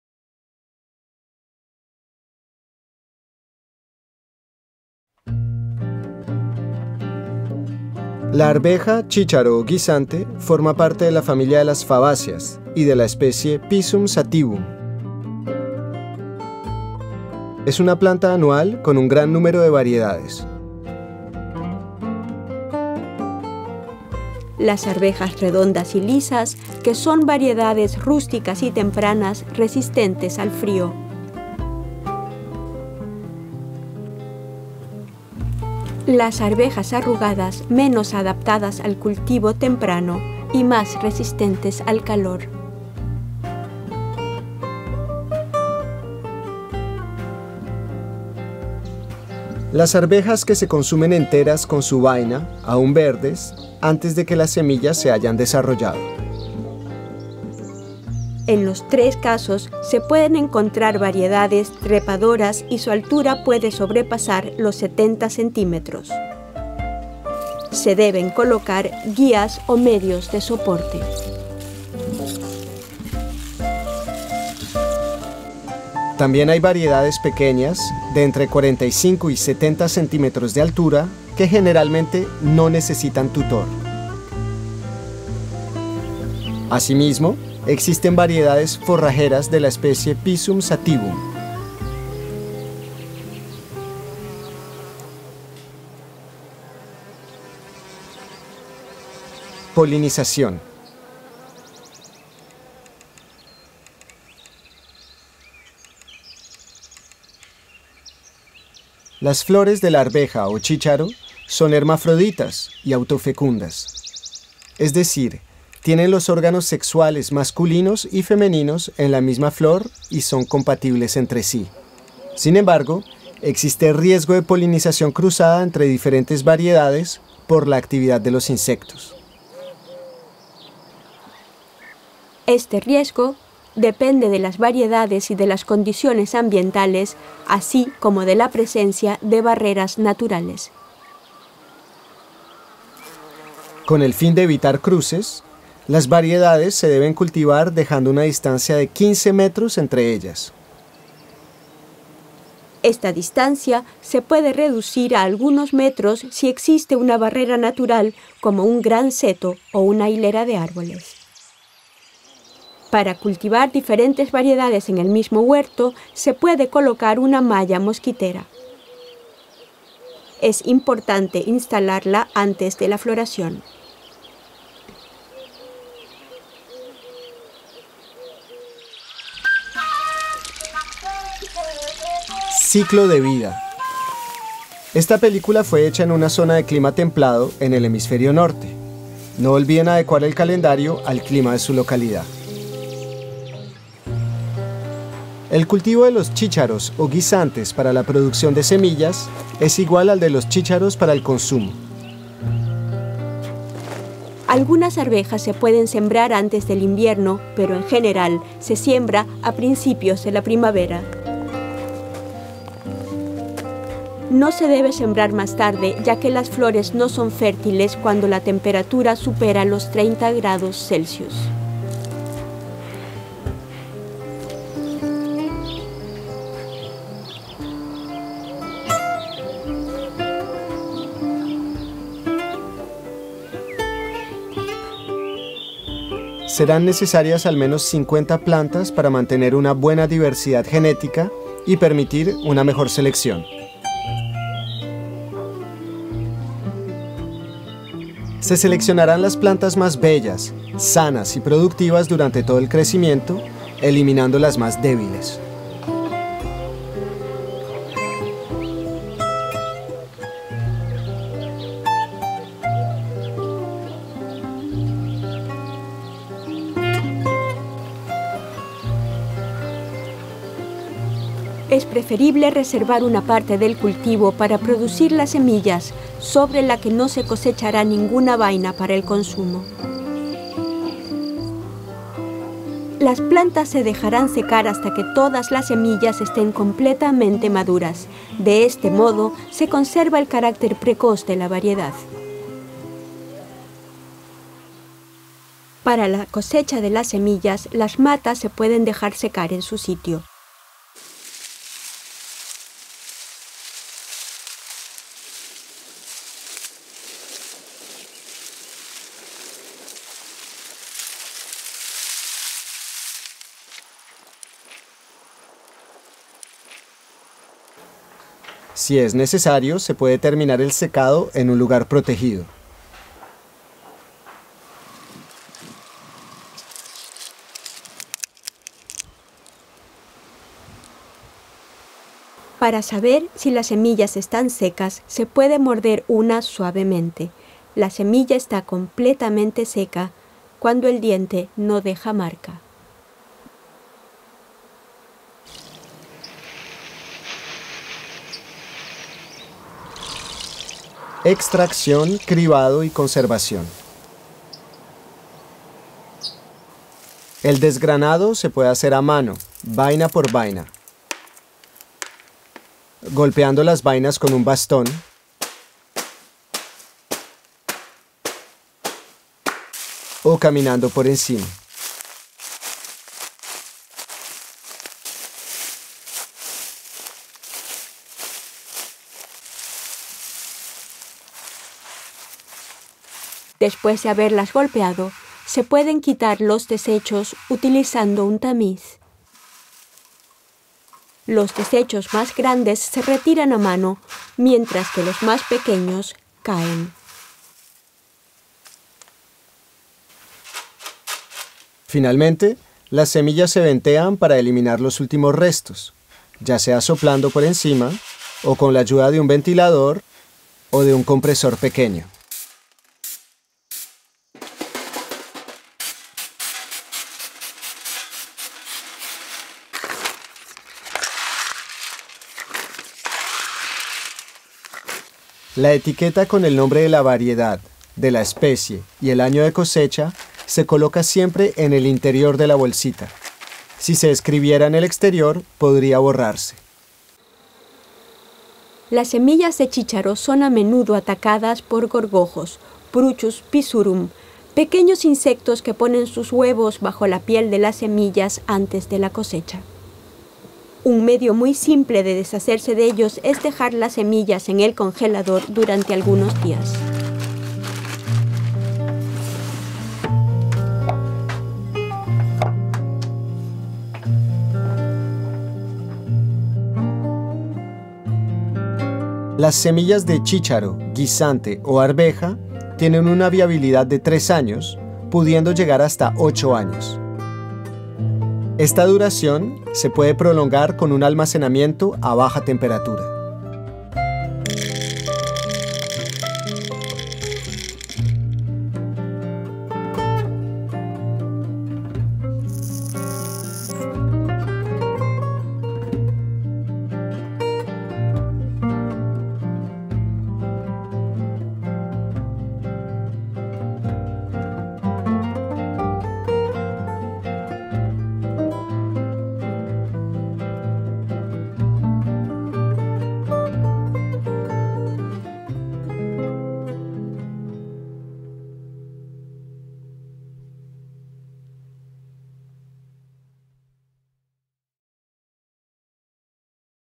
Cómo reproducir las semillas de arveja, chícharo o guisante
Gracias a este vídeo, podrás comprender su ciclo de vida, aprender a elegir y cultivar las plantas que producirán las semillas, cosechar, seleccionar y conservar las semillas. Siguiendo las explicaciones didácticas y las imágenes aprenderás paso a paso los gestos y trucos necesarios para reproducir tus propias semillas.